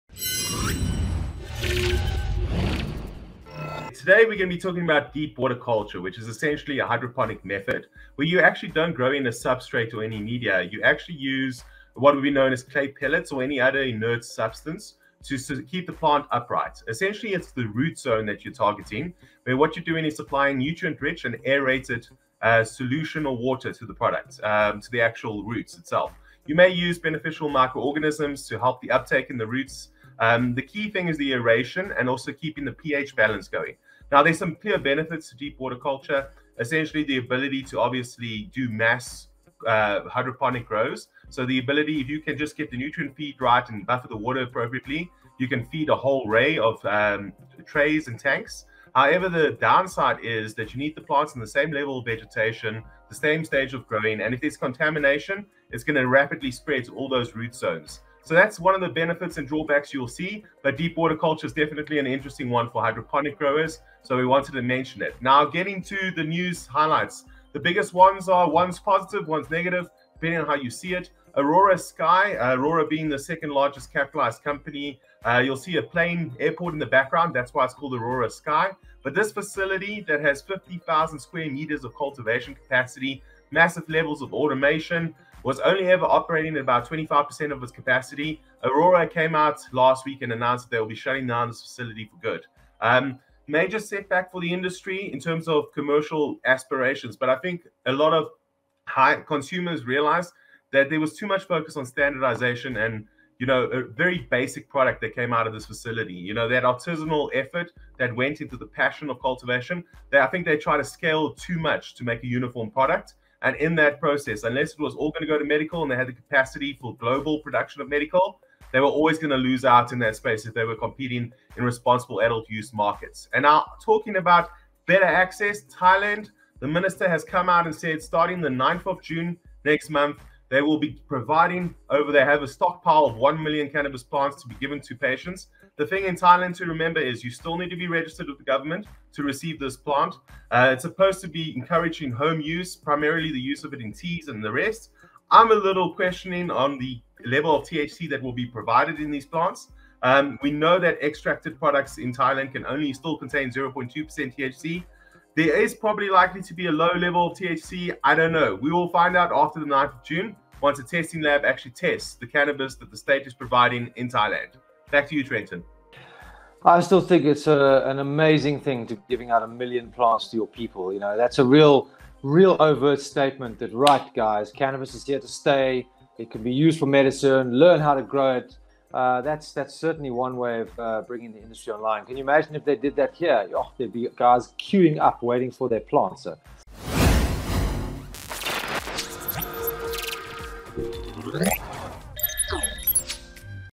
Aurora Cannabis closes Sky production - CannaSphere Shorts
On May 12, 2022 Aurora Cannabis announced the closure of its Sky facility in Edmonton, Alberta. The company had significant financial struggles in their most recent quarter, reporting over $1 billion net loss. The marijuana producer said they plan to keep the headquarters office open in Edmonton.